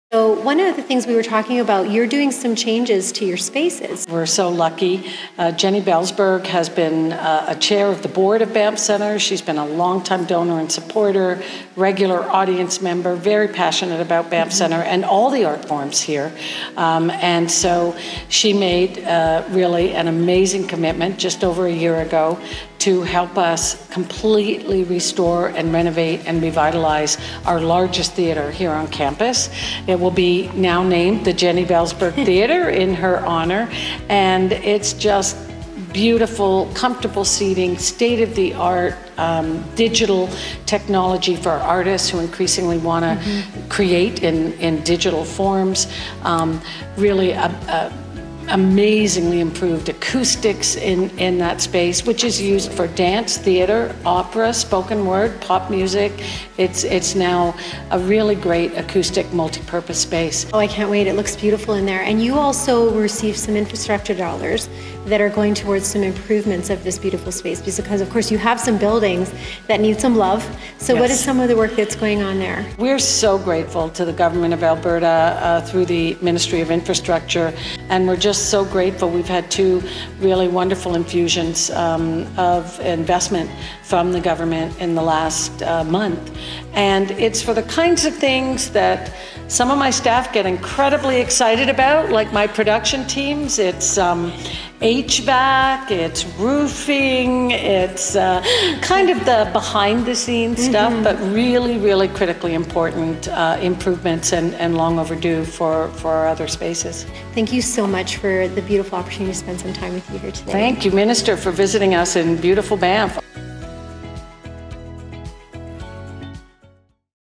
Banff Centre for Arts and Creativity
Thank you Janice Price for a tour of the
The Centre continues its role as a catalyst for creativity.